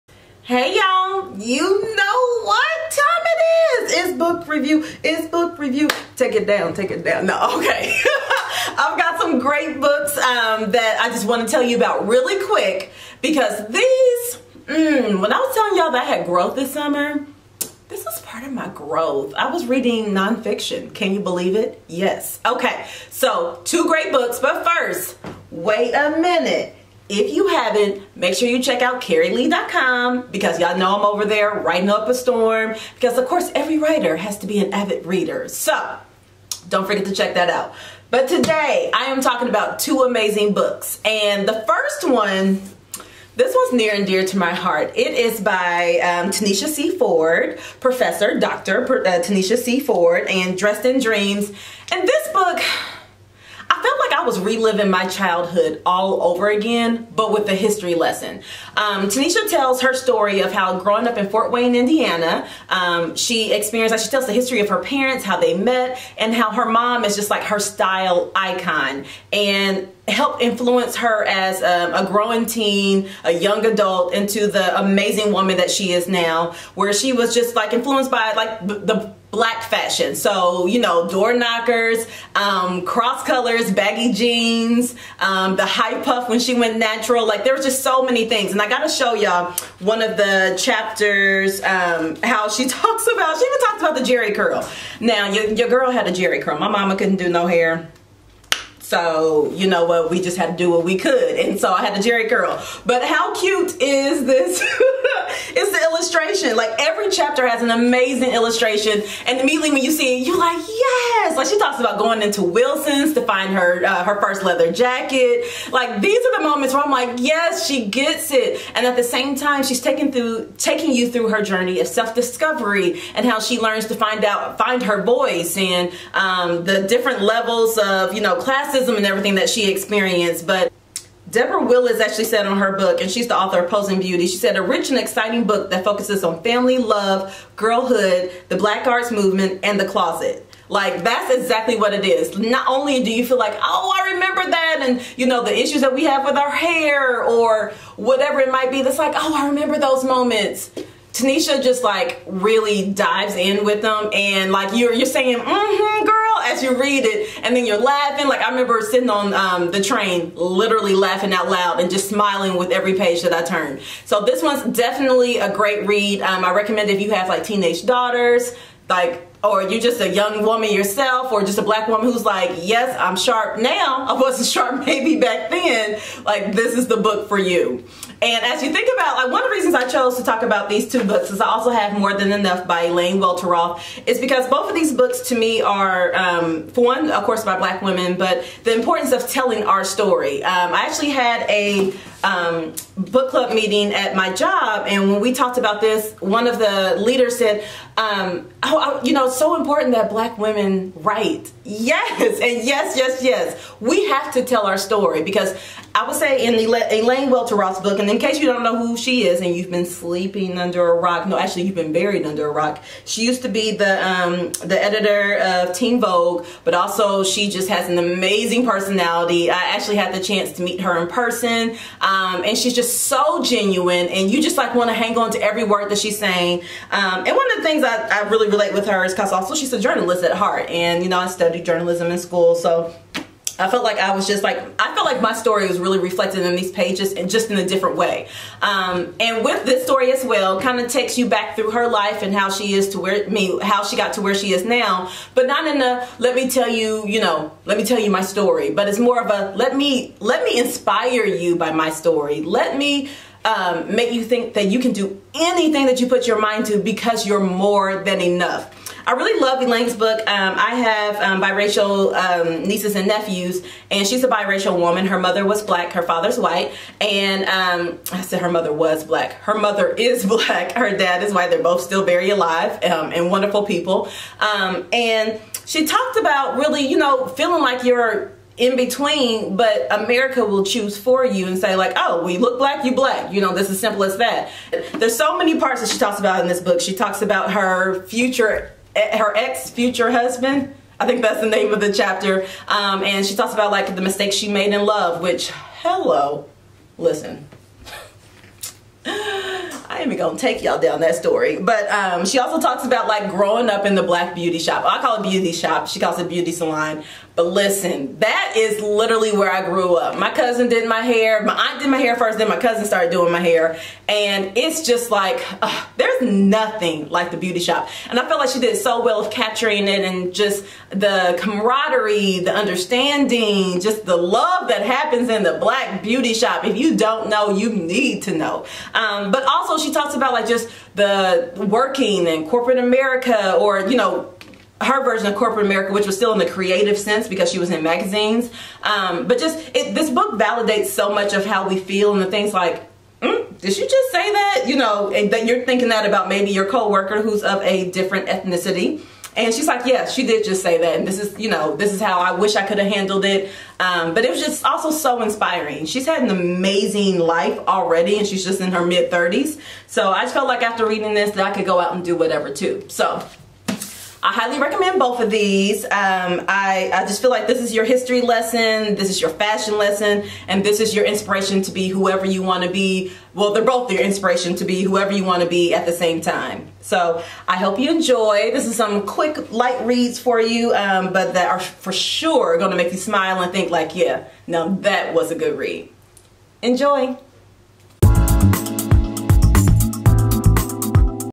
Fall Book Review: More than Enough by Elaine Welteroth
I review two inspiring non-fiction books by Elaine Welteroth and Tanish C. Ford. These are great book club choices for any woman.